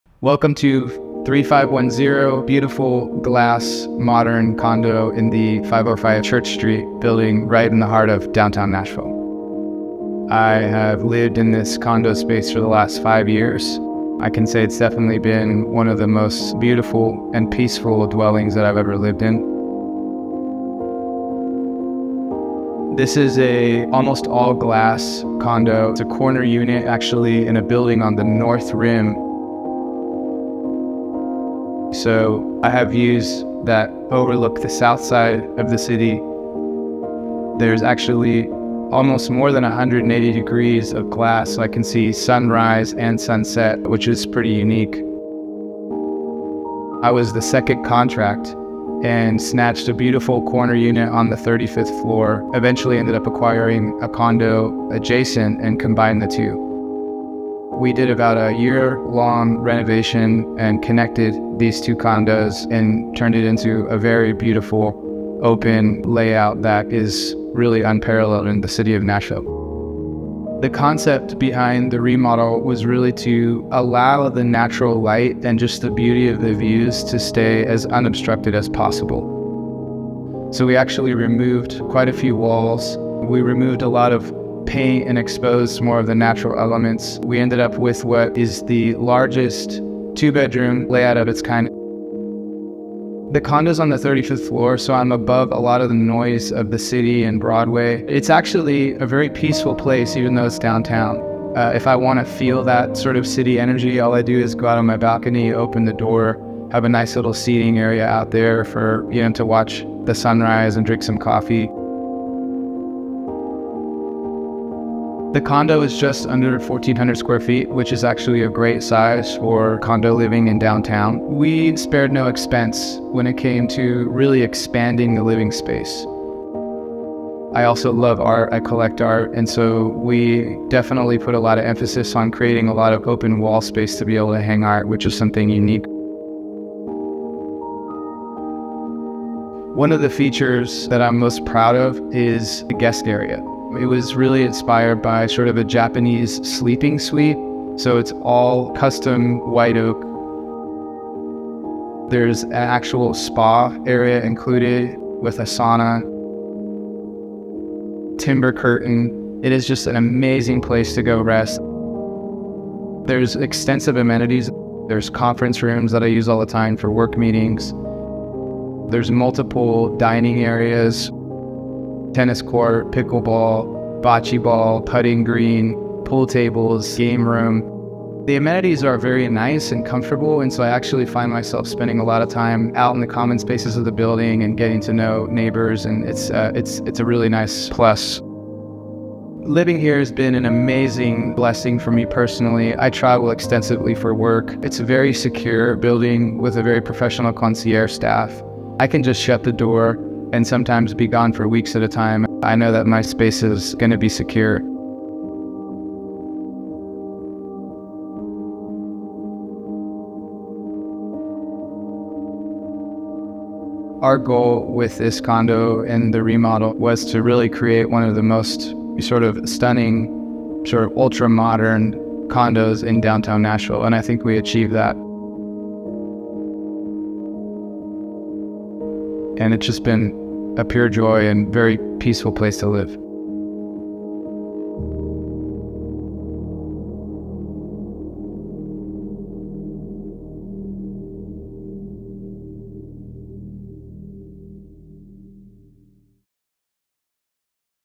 How a Young Executive Created Hidden Haven in a Downtown Nashville Tower : 515 Church Street #3510
An ultra-modern corner unit in the exclusive 505 tower with over 180 degrees of panoramic high-rise views overlooking Nashville’s iconic downtown, sunrise to sunset. This impeccable 35th-floor remodel offers contemporary elegance and luxury that inspires calm in the epicenter of the city. The owner spared no expense combining two condos to create an intelligent design complete with a custom office, white oak Japanese guest spa, infrared sauna, private south facing balcony, and a solar backup system. The 505 boasts a 24/7 concierge, state-of-the-art fitness, owner’s lounge and private dining, wine cellar, and must-see pools, cabanas, and tennis & pickleball courts! Includes not 1, but 2 hand-chosen, secure, deeded parking spots.